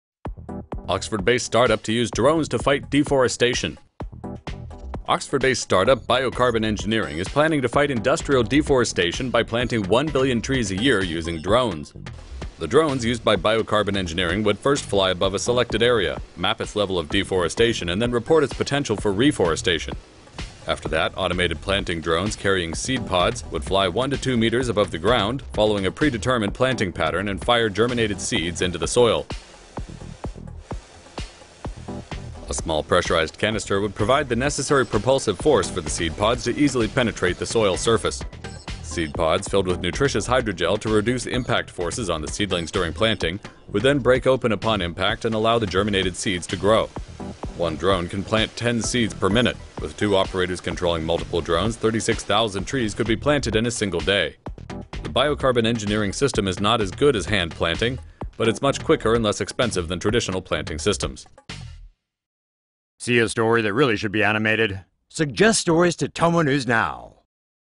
Seed-bombing drones to be deployed in fight to reforest the Earth with one billion trees per year
Oxford-based start-up Biocarbon Engineering is planning to fight industrial deforestation by planting 1 billion trees a year using drones.

Currently, 26 billions trees are cut down every year, while only 15 billions are replanted. The drones used by Biocarbon Engineering could solve the problem by making tree planting faster and cheaper.

In a video made by the company, Biocarbon Engineering explains that the drones would first fly above a selected area, map its level of deforestation and report back its potential for reforestation. Then, automated planting drones carrying seed pods would fly one to two metres above the ground, following a pre-determined planting pattern, and fire germinated seeds into the soil.

A small pressurized canister would provide the necessary propulsive force for the seed pods to easily penetrate the soil surface. The seed pods, filled with nutritious hydrogel to reduce the force of the seed’s impact with the soil, would then break open upon impact and allow the germinated seeds to grow.

According to the Independent, one drone is able to plant 10 seeds per minute. With two operators controlling multiple drones, 36,000 trees could be planted in a single day.

Biocarbon Engineering hopes to have the system fully working by the end of the summer.

According to the Independent, the drone system is not as good as hand-sowing, but it’s much quicker and less expensive than traditional planting systems.